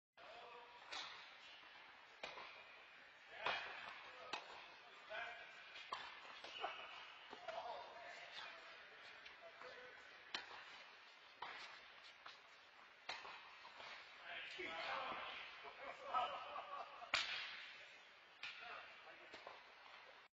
Got a racquet on it
In this video I had to turn my back to avoid getting hit by a smash and miraculously I ended up getting my racquet on it to extend the point and eventually win the point!!!